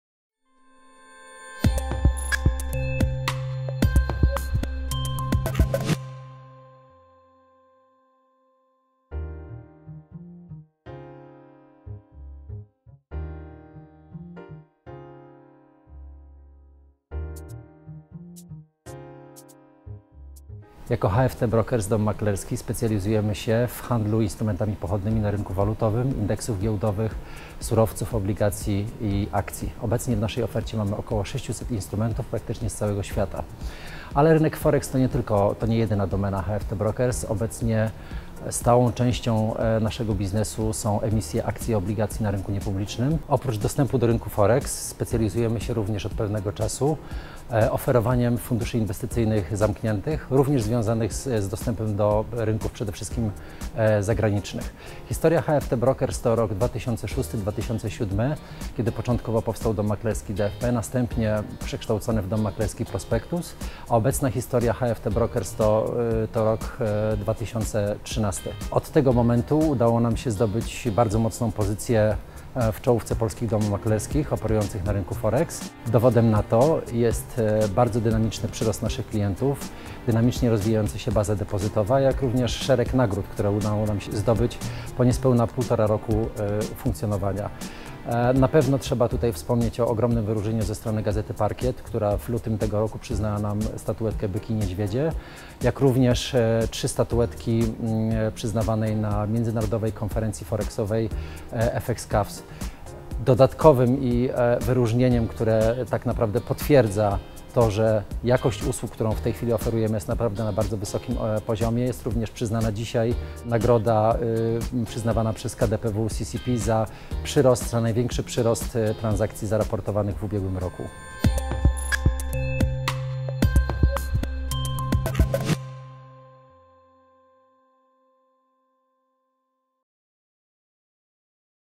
Uroczystość Wręczenia Nagród Za Osiągnięcia w 2015 roku: Tomasz Uściński, HFT Brokers DM S.A.
Uroczystość wręczenia nagród uczestnikom rynku kapitałowego, która odbyła się 12. maja 2016 roku była dobrą okazją dla Prezesa Zarządu HFT Brokers Dom Maklerski S.A. do przedstawienia nam historii Spółki.